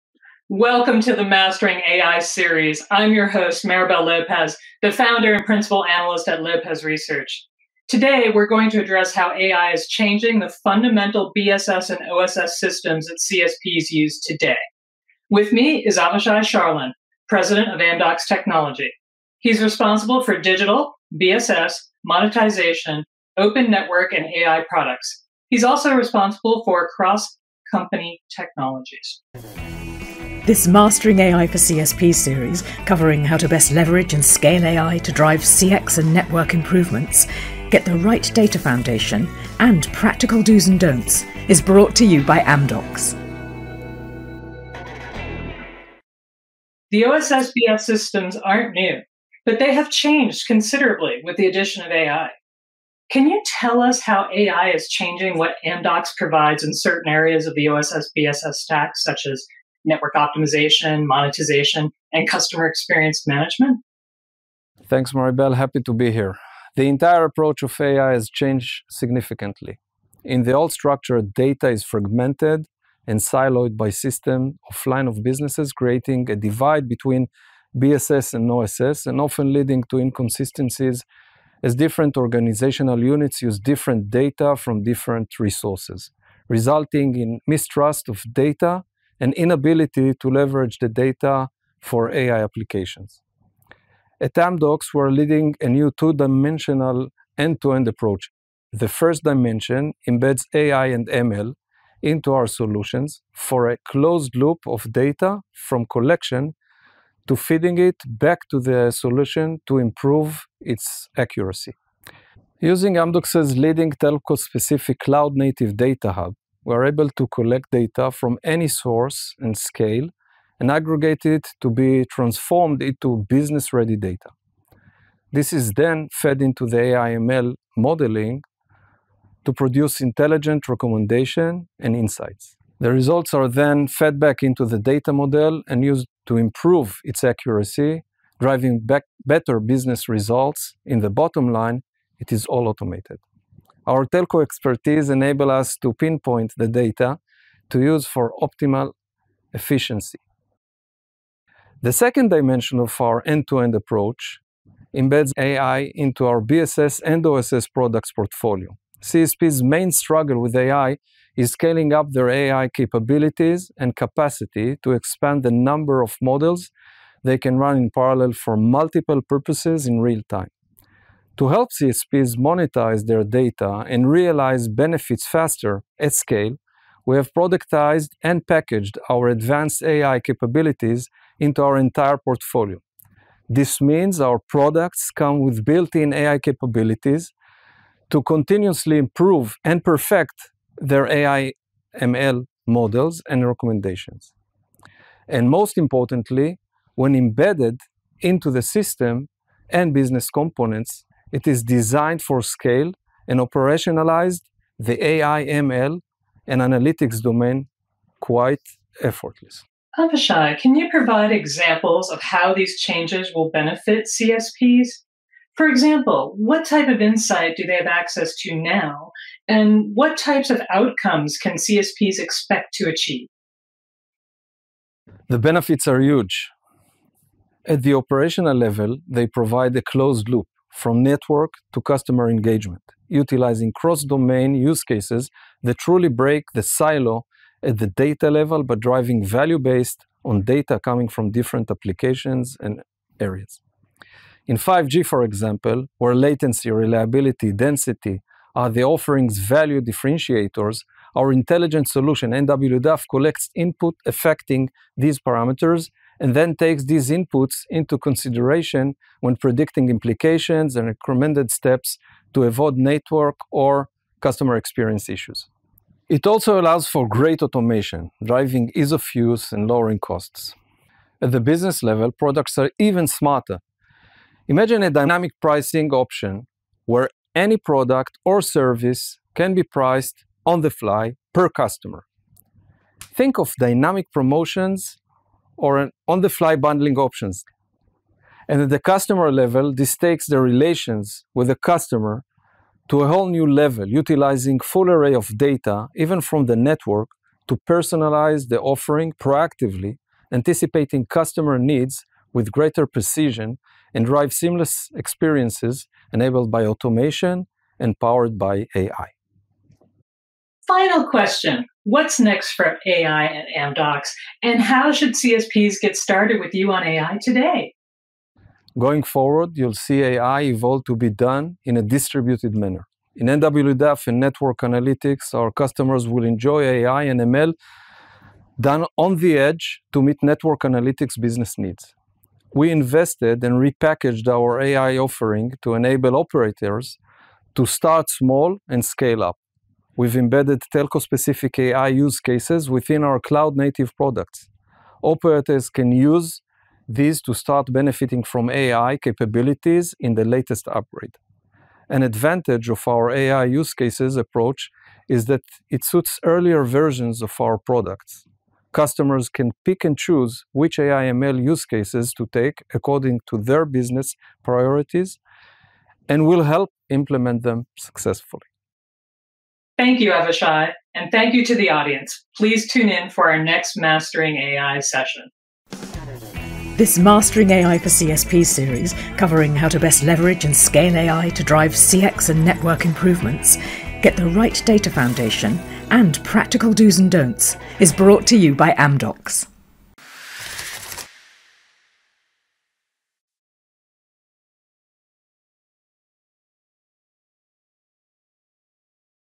Episode #5: How AI-enabled OSS/BSS empower transformation.
This AI masterclass for CSPs covers how to best leverage & scale AI to drive CX and network improvements, Get the right data foundation, and practical Do’s & Don’ts!

In the fifth episode, leading analyst Maribel Lopez is joined by Avishai Sharlin, president of Amdocs Technology, to discuss how AI is changing the fundamental OSS & BSS systems that CSPs use today to help CSPs monetize their data and realize its benefits faster, at scale